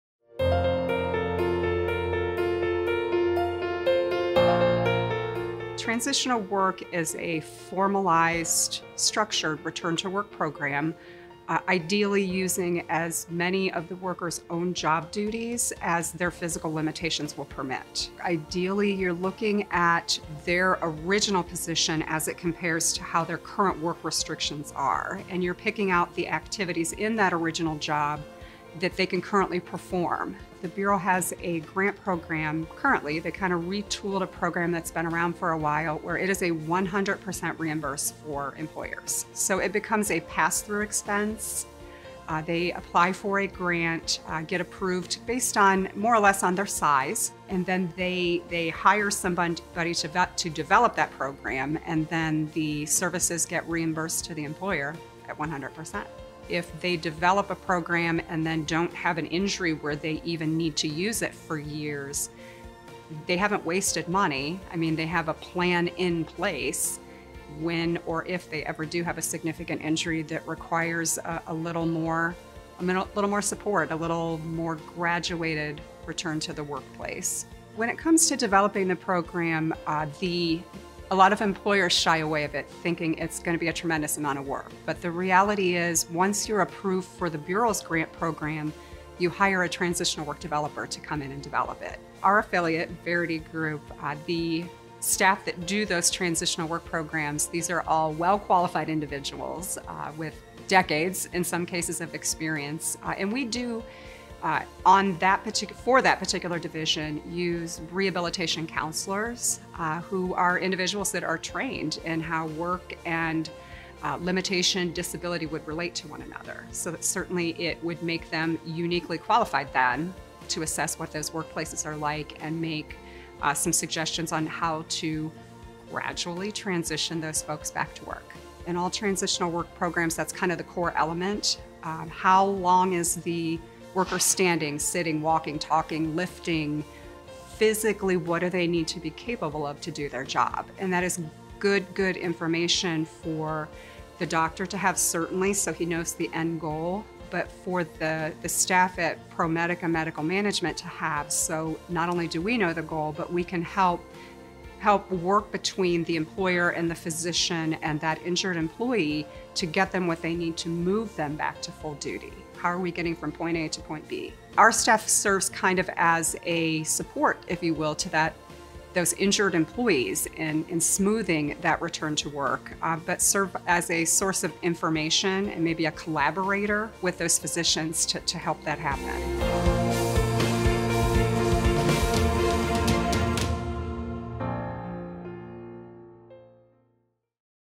What is Transitional Work?
ProMedica Medical Management Account Executive, Denise Nutter, explains to Ohio employers what transitional work programming is and the importance of proactively setting up a program for their employees.

At ProMedica, our Mission is to improve your health and well-being.